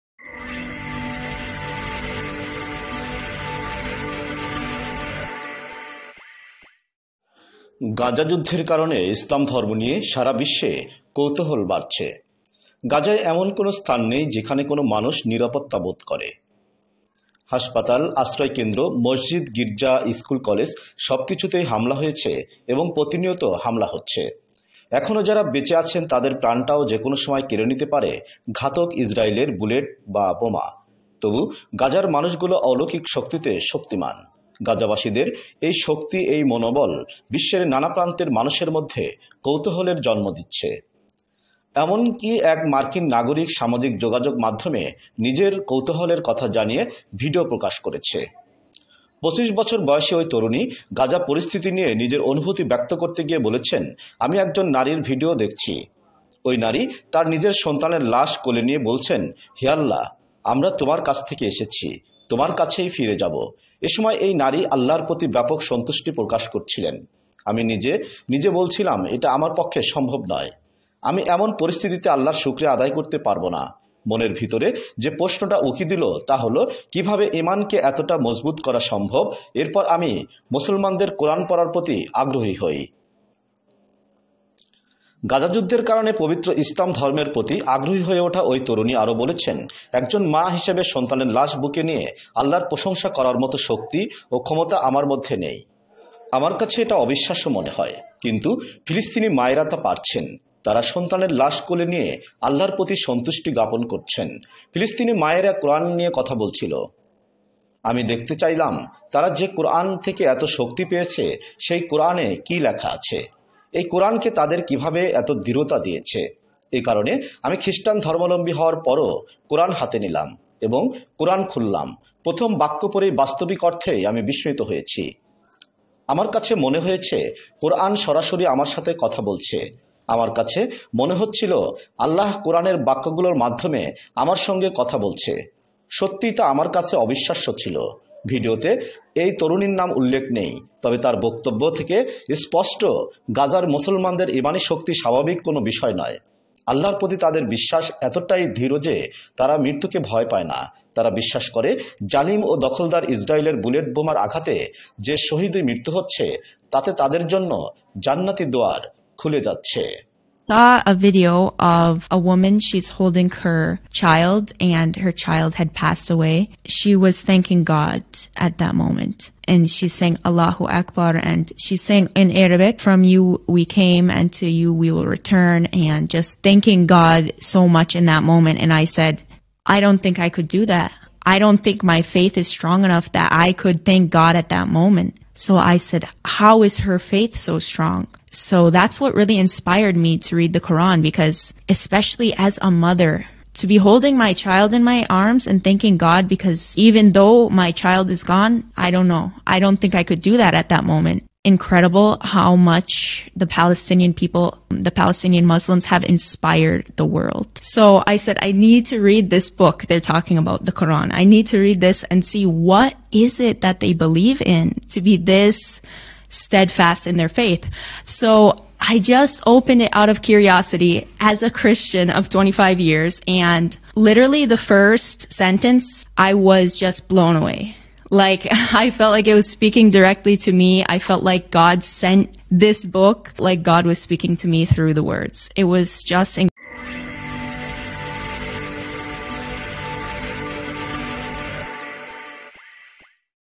গাজা যুদ্ধে ফিলিস্তিনী মায়েদের ঈমানী দৃঢ়তা হতভাগ করছে বিশ্ববাসীকে ( মার্কিন তরুনীর ভিডিও ভাইরাল)
ফিলিস্তিন#
গাজা উপতক্যা#
ইসরাঈল#
ফিলিস্তিনী মা#
ঈমানী র্দঢ়তা#
মার্কীন তরুনী#
ফিলিস্তিনী মায়ের ঈমানী চেতনা#
ইসরাঈলী আগ্রাসণ#